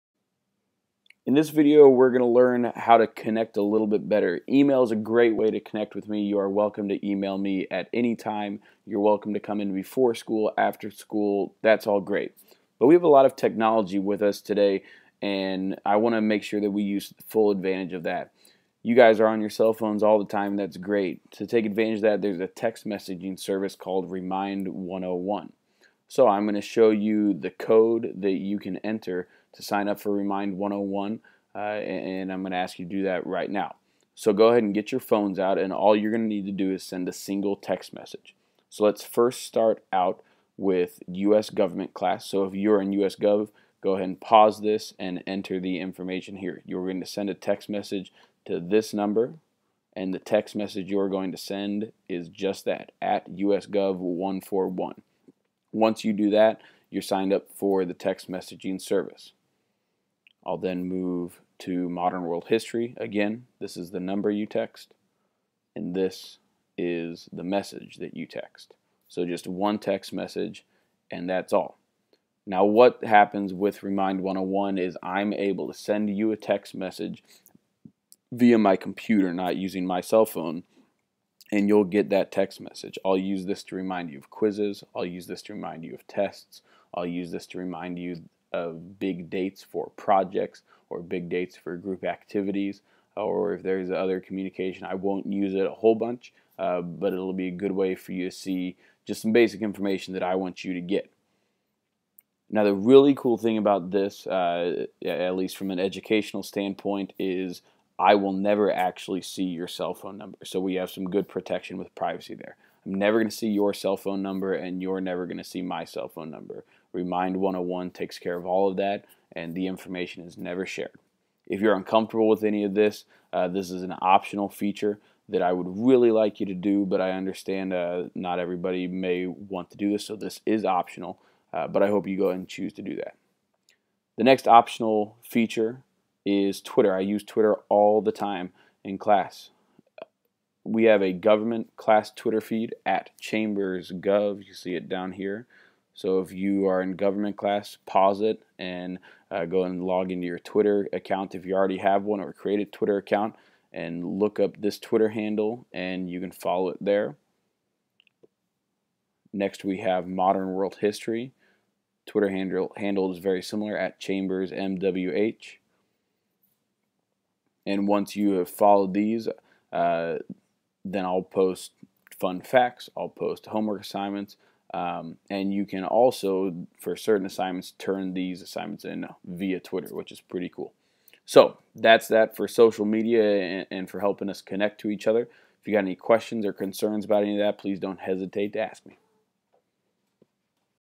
Chambers Tech Module Video 6 - Social Media
Video 6 in the technology module discusses safe use of social media in my class specifically, and on a more general level as well.